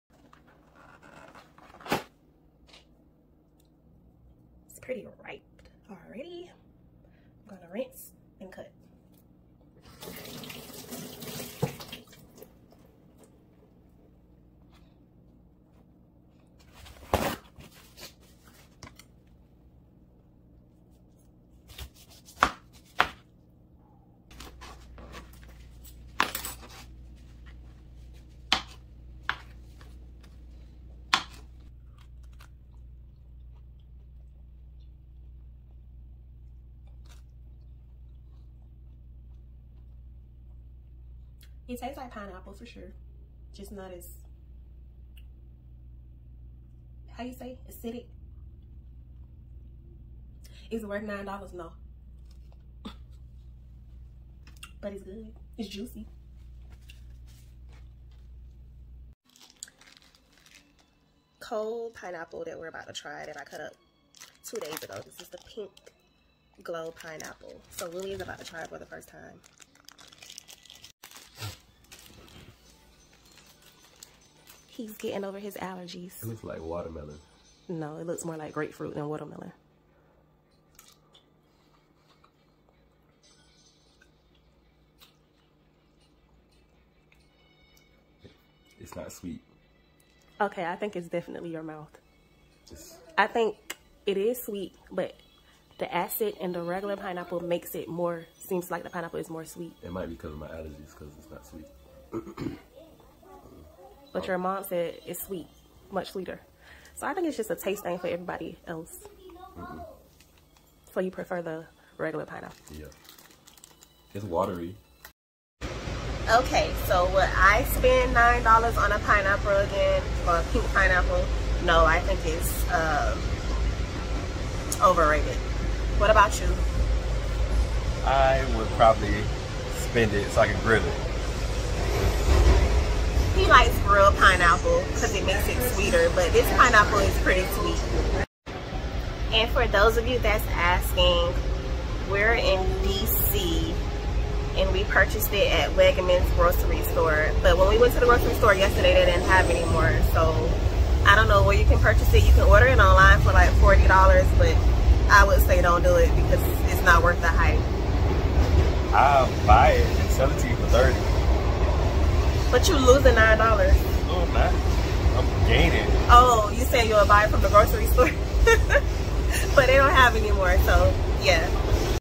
Pinkglow Pineapple Review🍍✨️
We wanted to see what the hype was about! is the pink pineapple really that good?! Apparently, it's extremely hard to find in stores, and it's expensive.

Did you know that it is also illegal to grow? Delmonte owns the rights to the Pinkglow. All pineapple heads are removed before boxing to prevent others from harvesting!

Have you tried the pink pineapple?